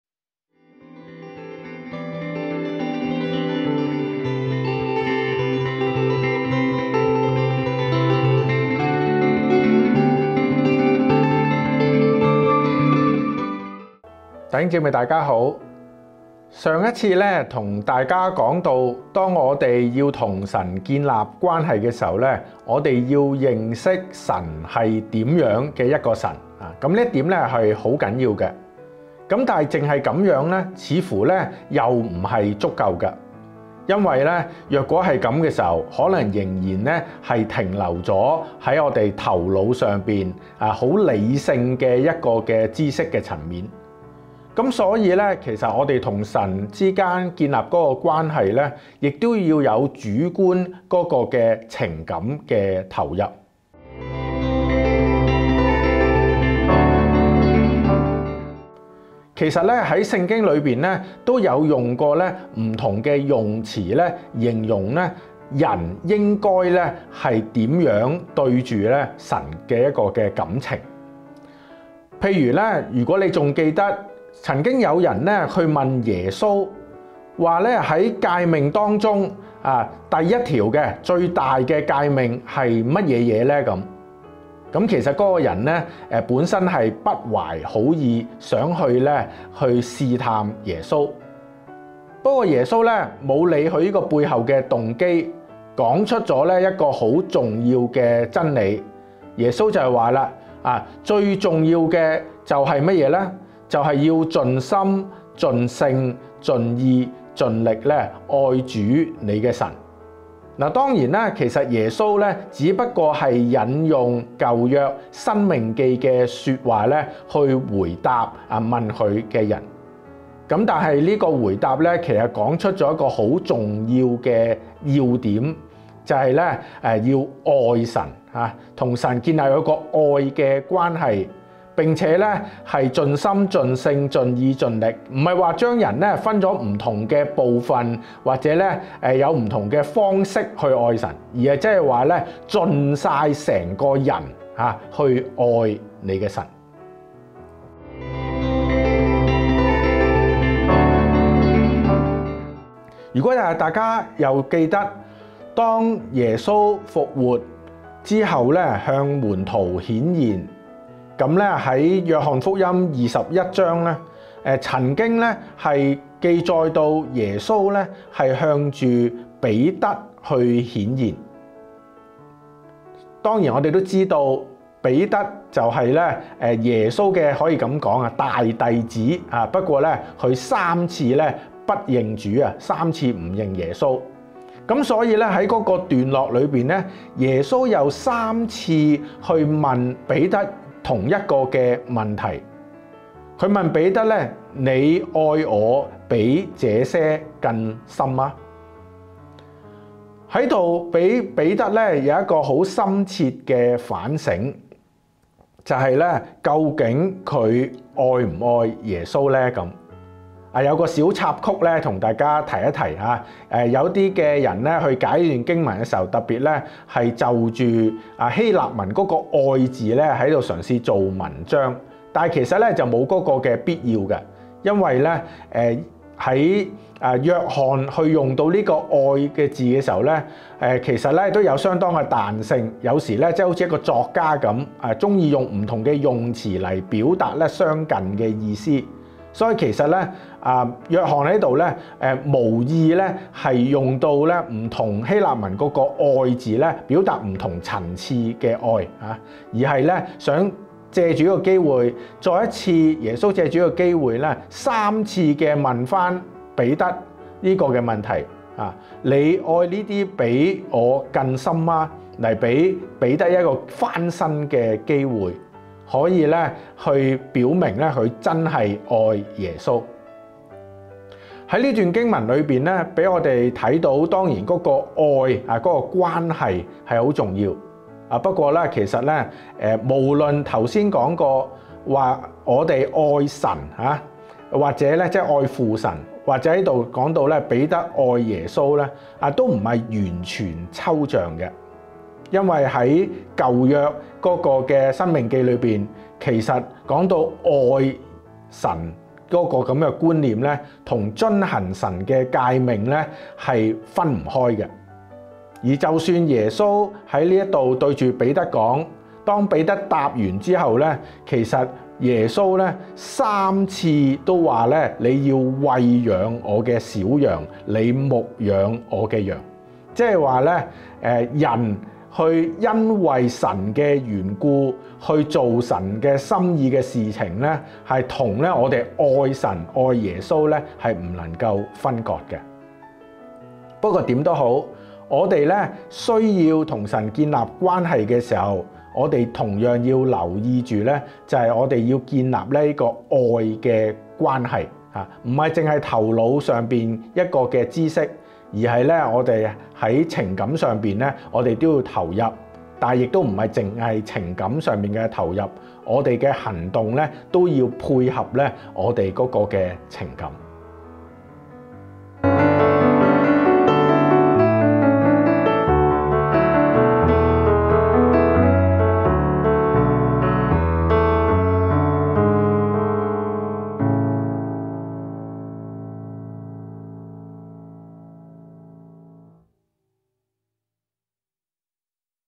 11.2 愛主你的神
我們都明白神對我們的大愛，那我們回應祂的愛夠【盡】嗎？

何俊華牧師的「齊心承傳福音，銳意建立門徒」
網上信仰分享系列 -

WhatsApp Broadcast list
▪️ 將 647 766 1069 加到你的電話聯絡人
▪️ 向我們發一個WhatsApp短訊：「姓名 訂閱」